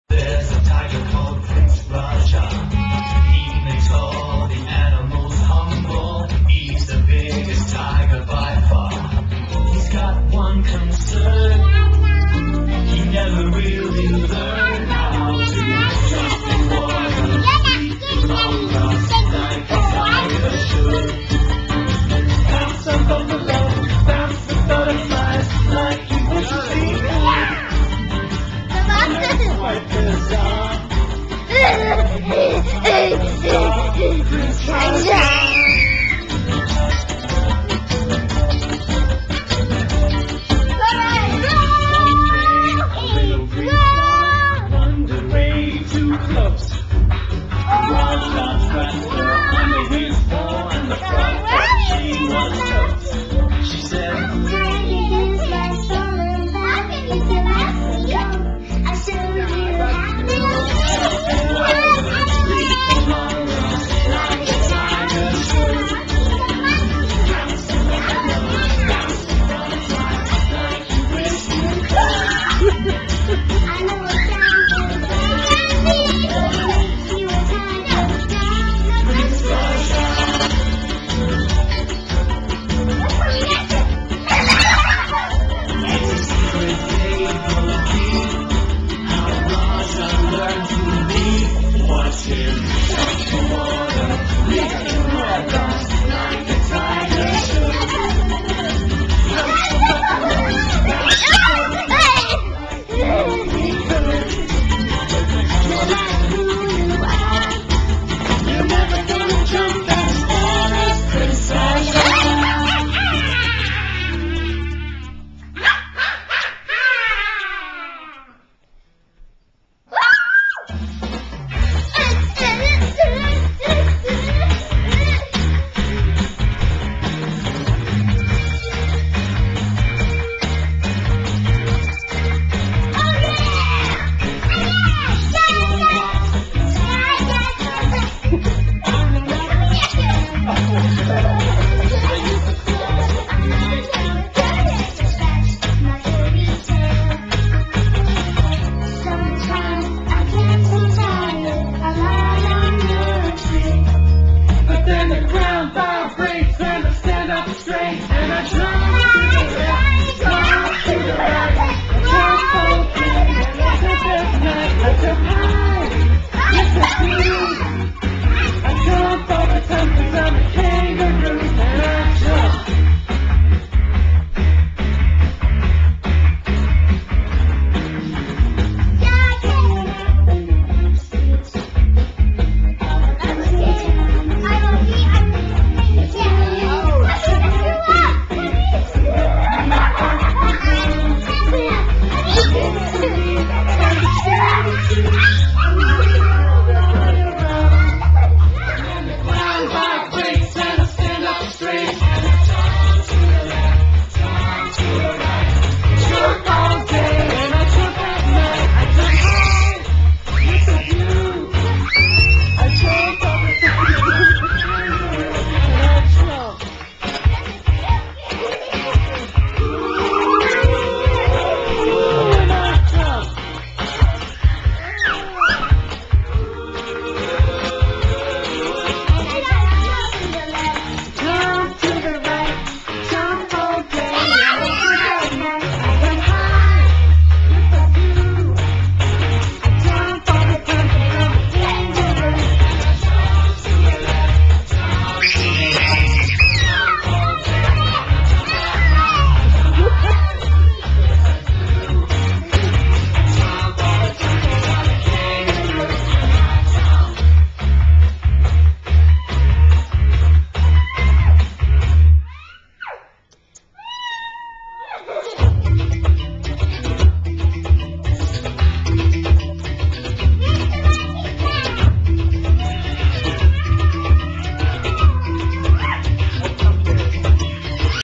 WE LOVE BINDI!
PLAYING WITH UNCLE STEVE AND LISTENING TO BINDI